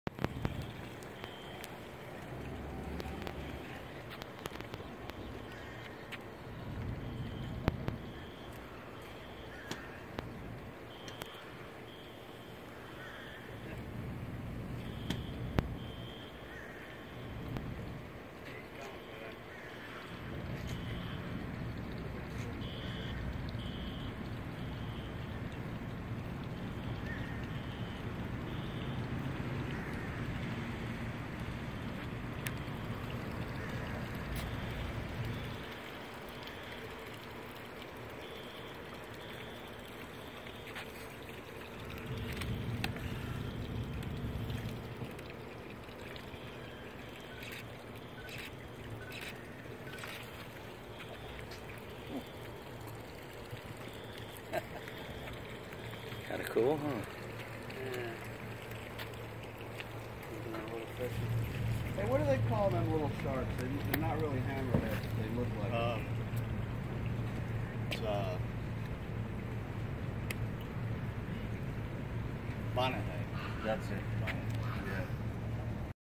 Bonnethead shark
Photographer notices a fin breaking the surface and captures a marine menace cruising the waters of the harbor. (Bonnetheads eat shrimp and scallops and grow to about four feet. This one is about nine inches long and is estimated to be only a few days old. Related to the hammerhead.) Filmed at Clearwater Harbor Marina.